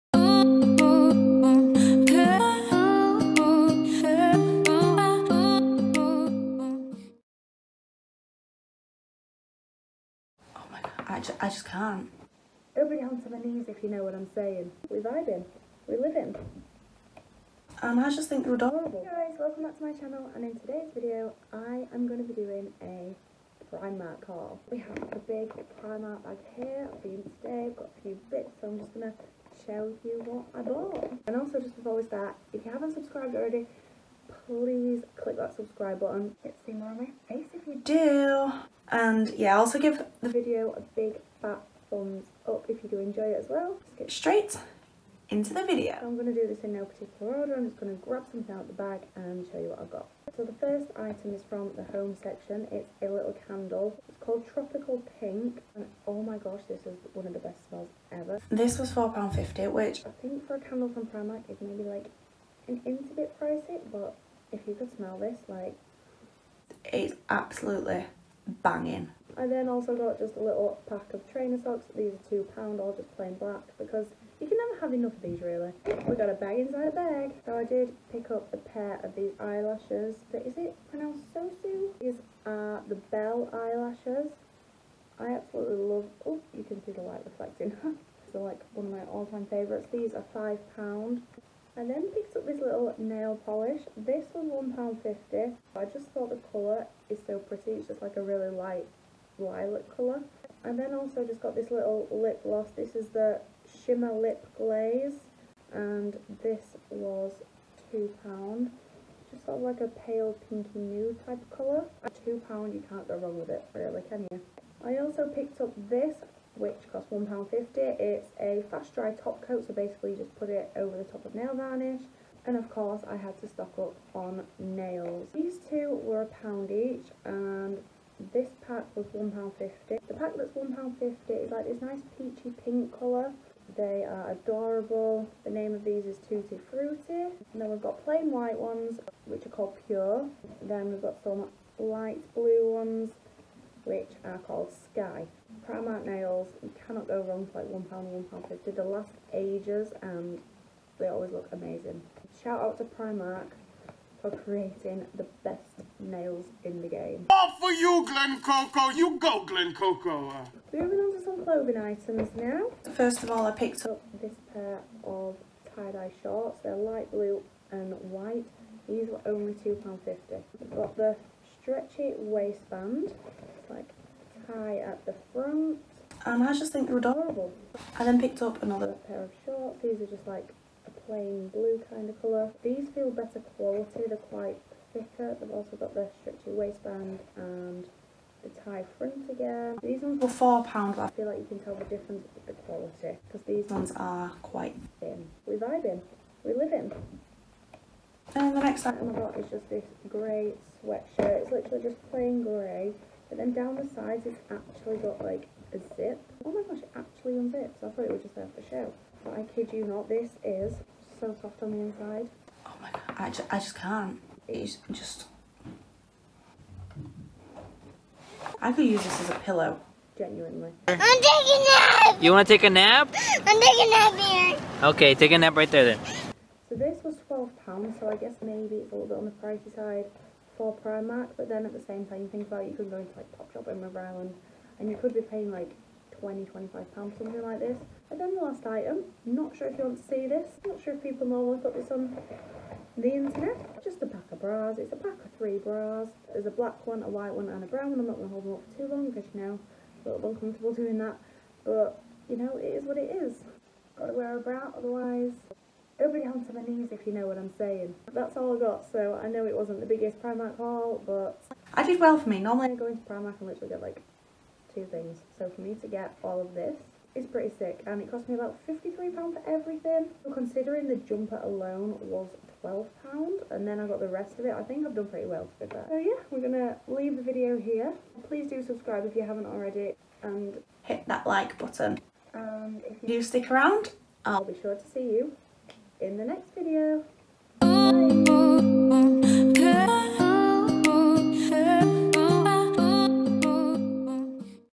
summer 2020 primark haul! first one since lockdown | hey! it's georgia
Everything i bought when i took a trip to primark, July 2020!
(Clothes, home, beauty, etc...)

- apologies for the terrible sound in this video! -

Upload schedule: Sunday's at 5:30pm and the occasional other day...